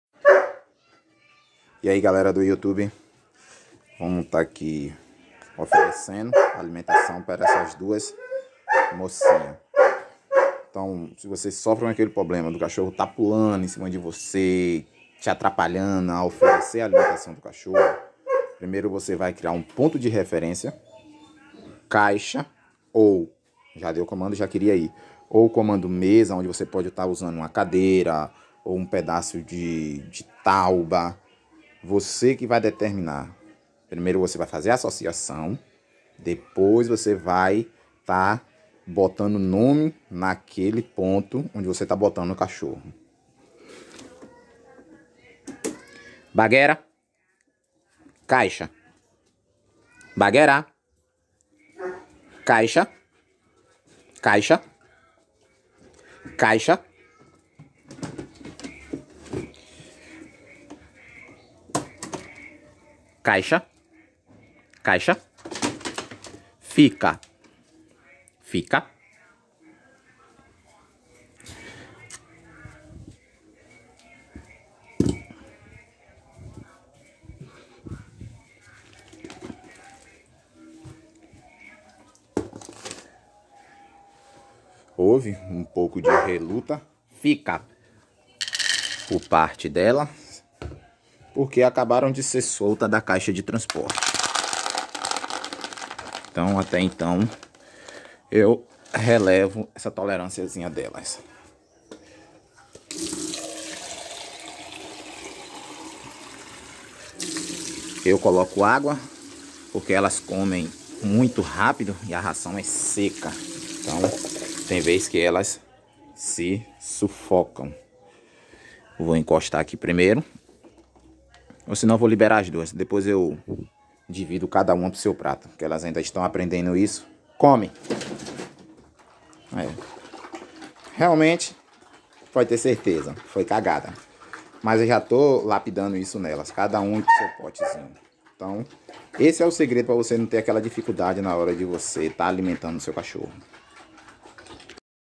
comando limitador na hora da alimentação
nesse vídeo eu mostro como vc deve agir na hora de oferecer a refeição do seu cachorro